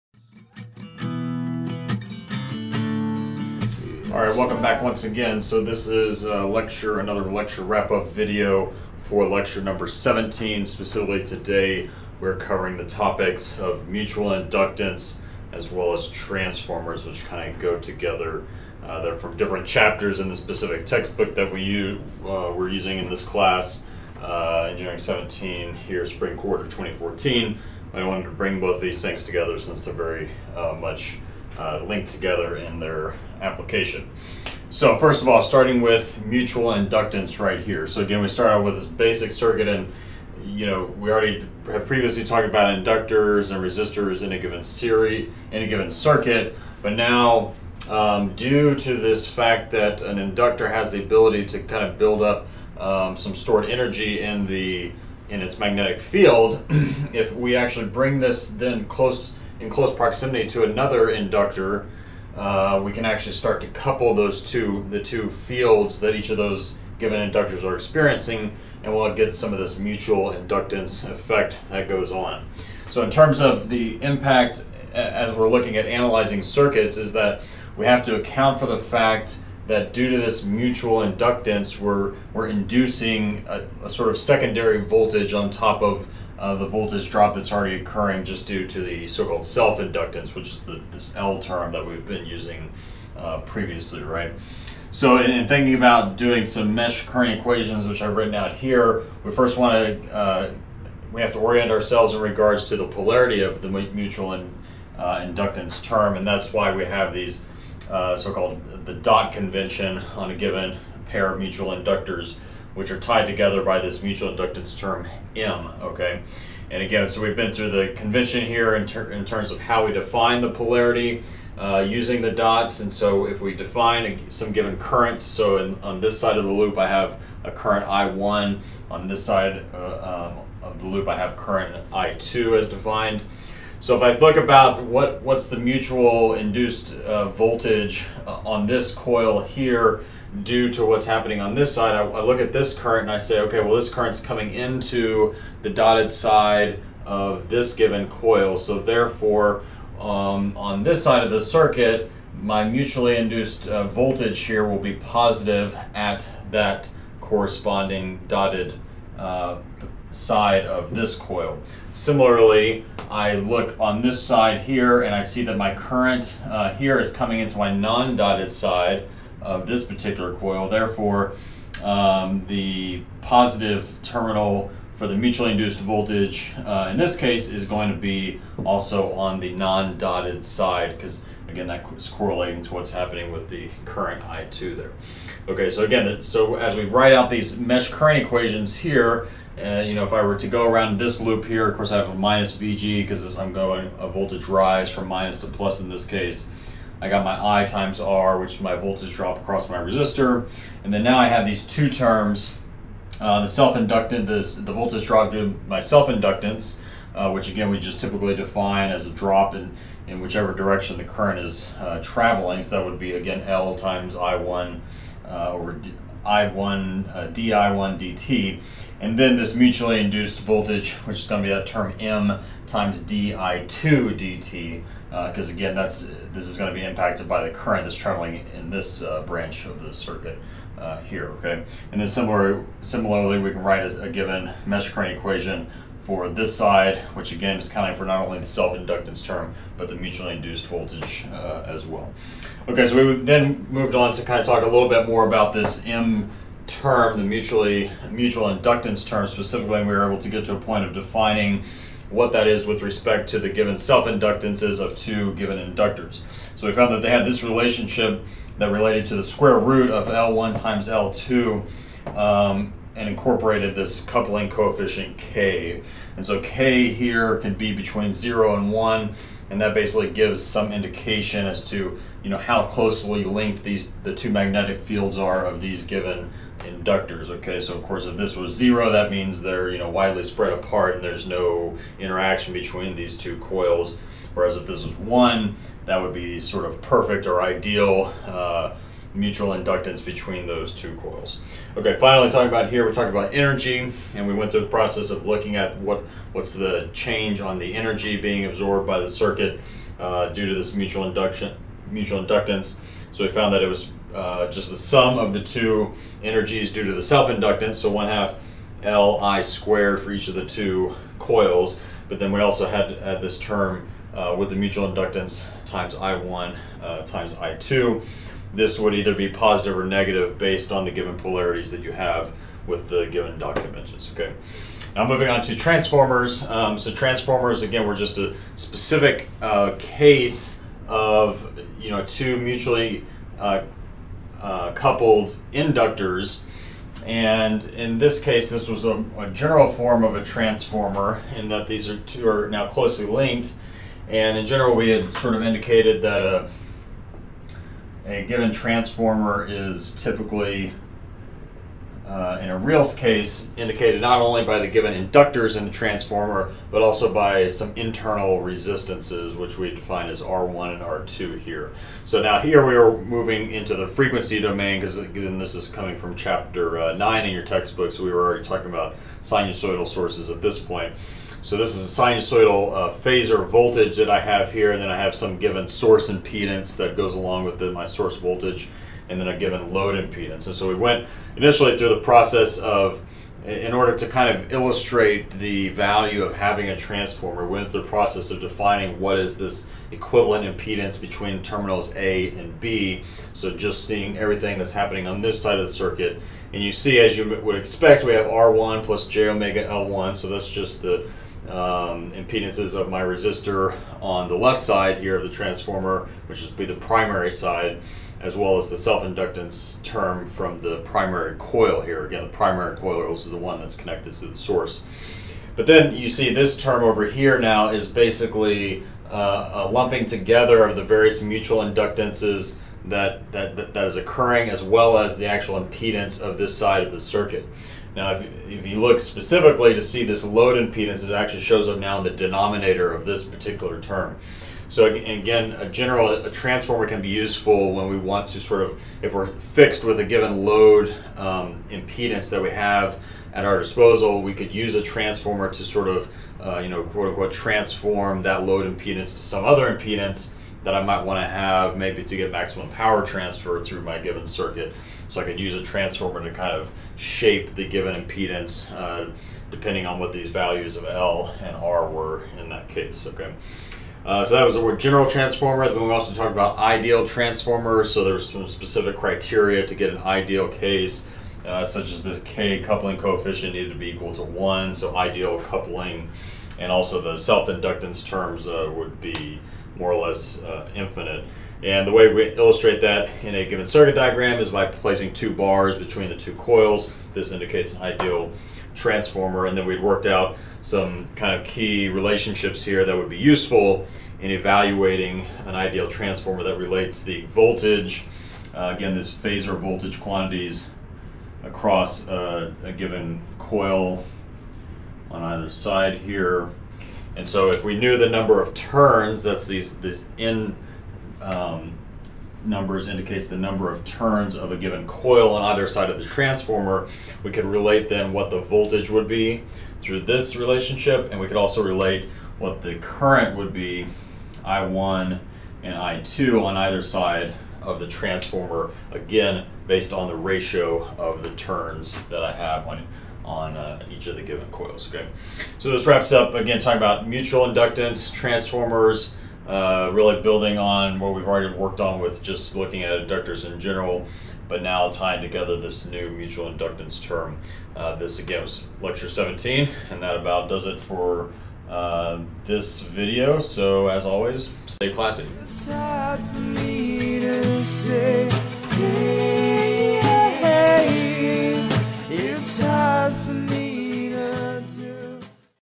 Circuits I: Lecture 17 Wrap-Up: Mutual Inductance & Transformers
This video provides a wrap-up of Lecture 17, which covered mutual inductance and transformers.

ENG17: Circuits I, Sec. 2, Spring 2014
Instructor: Dr. Kyle Montgomery